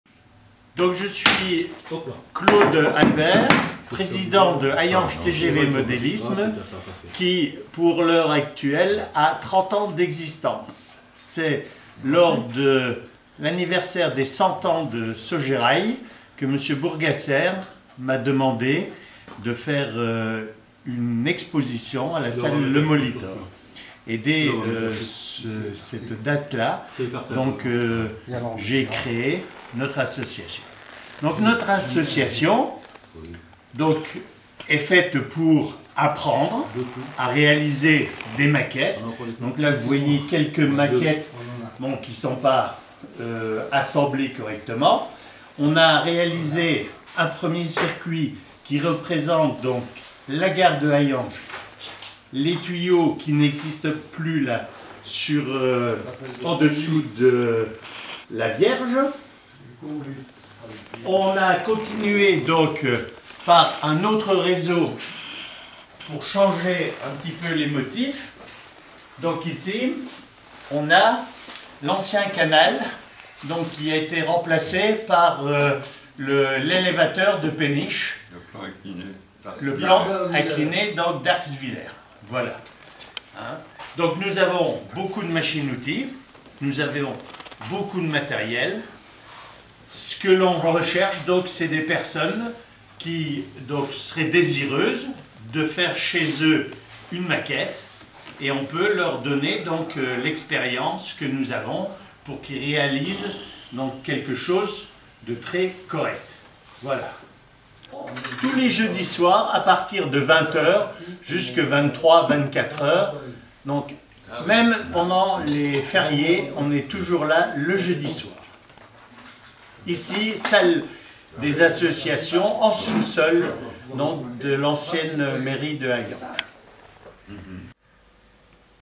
Claude ALBERT Hayange TGV Modélisme 2024
Le président Claude ALBERT de Hayange TGV Modélisme vous parle de son association et vous donne rendez-vous à leur local si vous êtes également passionné de Modélisme